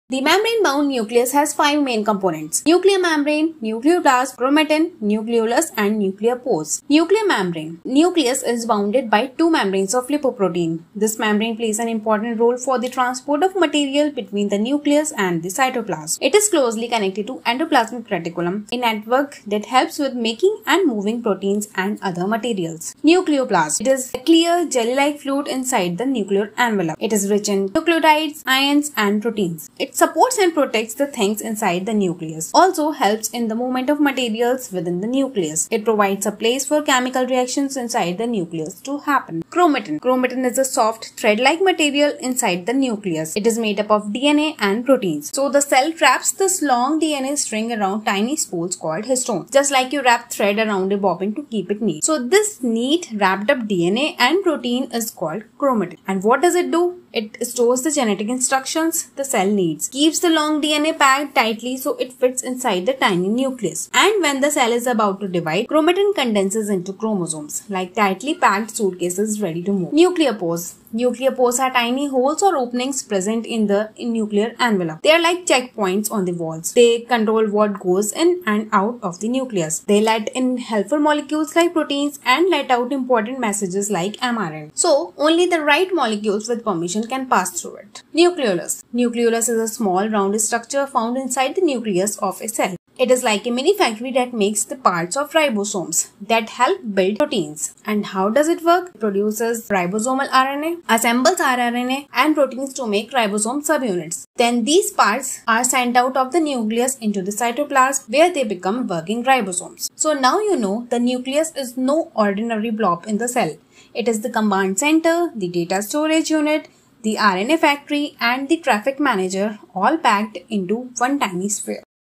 5 Key Parts of the Nucleus You Must Know
5 Key Parts of the Nucleus You Must Know

The nucleus is a membrane-bound organelle found in eukaryotic cells that stores the cell’s genetic material (DNA) and controls all cellular activities, including growth, metabolism, protein synthesis, and cell division.